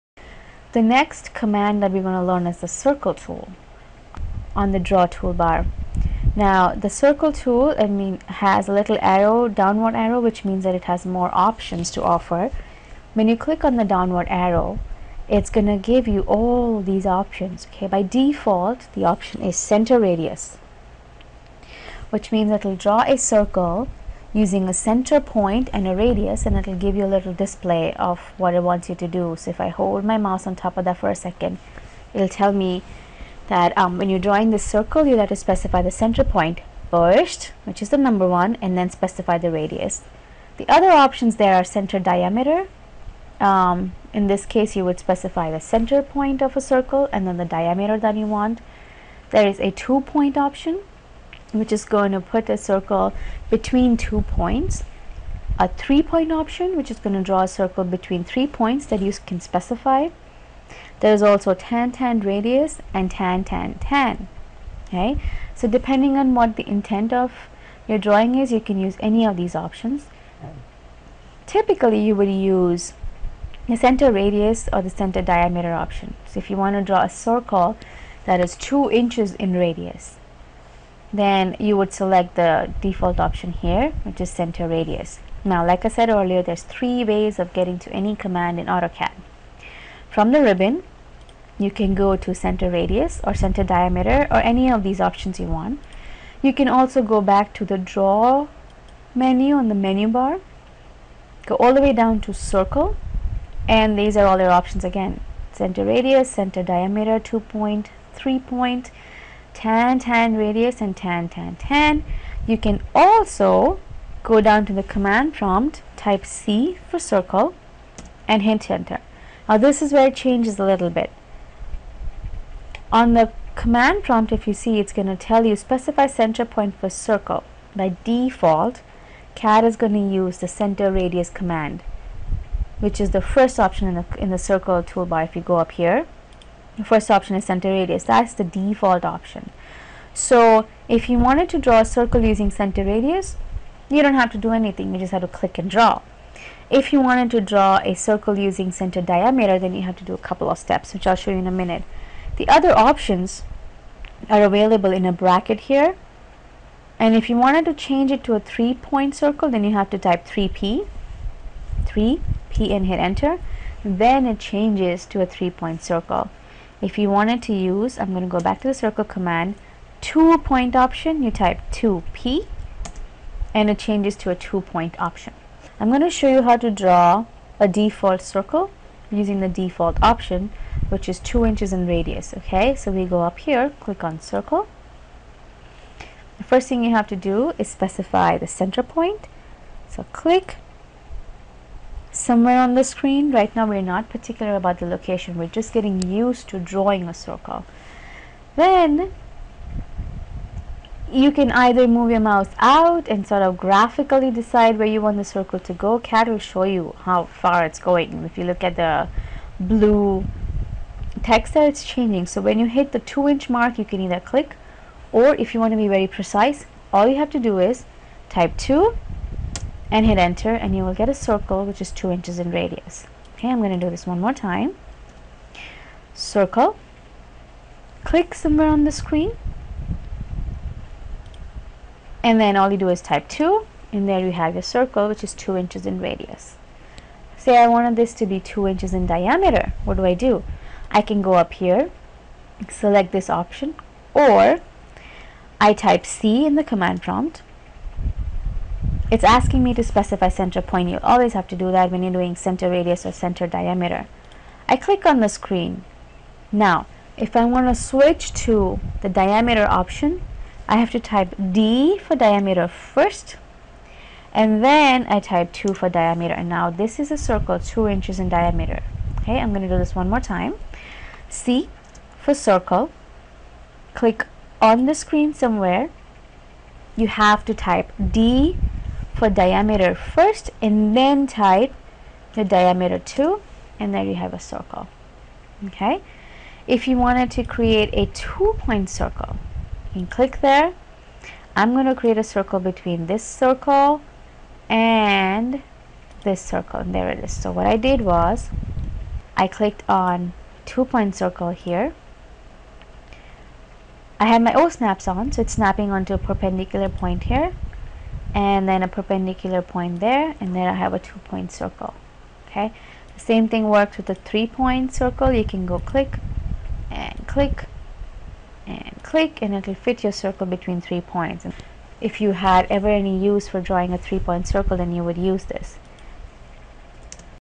AutoCAD 2012 Beginner Master in 3D/2D #5 Circle
View Now All Tutorials for this course:
AutoCAD 2012 Beginner Master in 3D/2D

The next command that we are going to learn is the Circle tool on the Draw toolbar. Now the Circle tool has little downward arrow which means that it has more options to offer. When you click on the downward arrow, it is going to give you all these options.

By default the option is Center, Radius which mean that you draw a Circle using a center point and the radius and that will give you a little display of what it wants to do, if I hold my mouse on top of that It tells me that when you are drawing a Circle, you have to specify the center point first which is the number 1, and then specify the radius. SEE... Jobboj.com